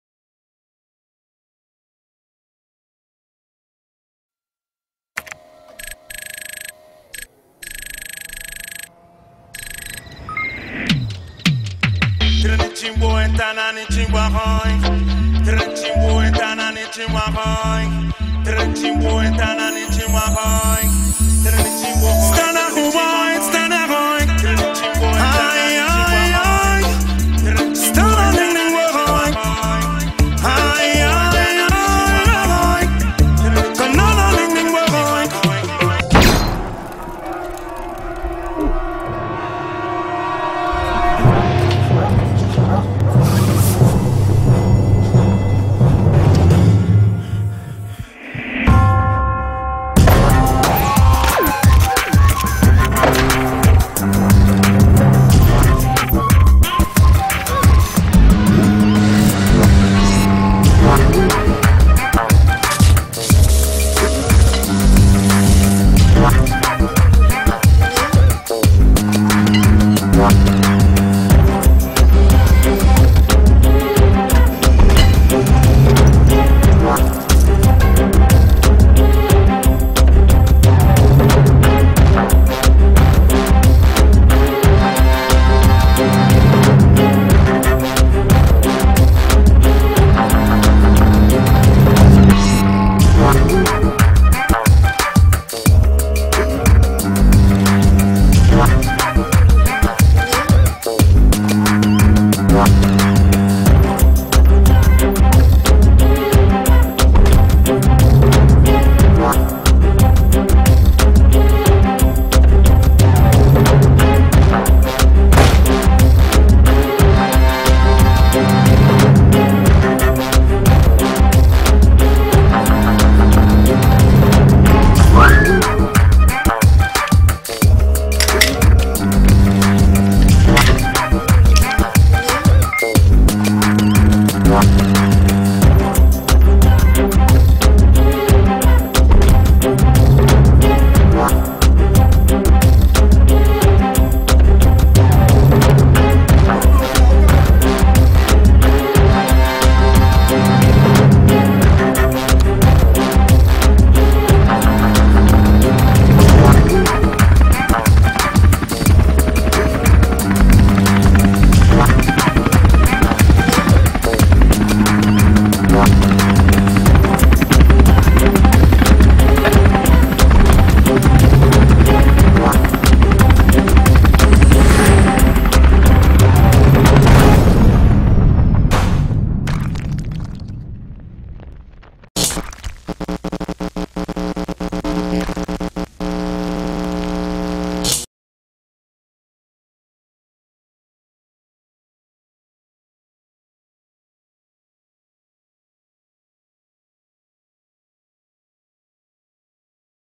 PORKFISH Show reel
Vidéo clip réalisé en pâte à modeler . PORKFISH